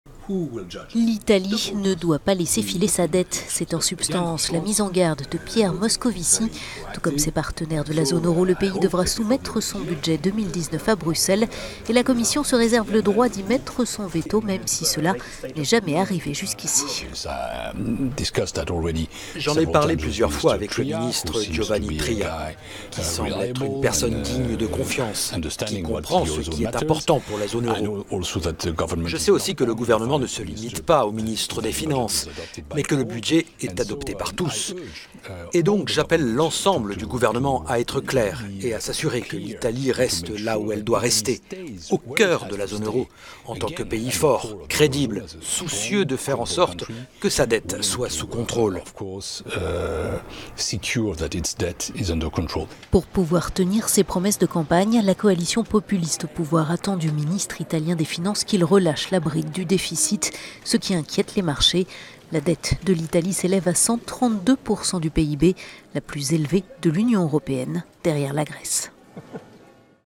La mise en garde de Moscovici à l'Italie
Le commissaire européenne l'appelle à ne pas laisser filer sa dette…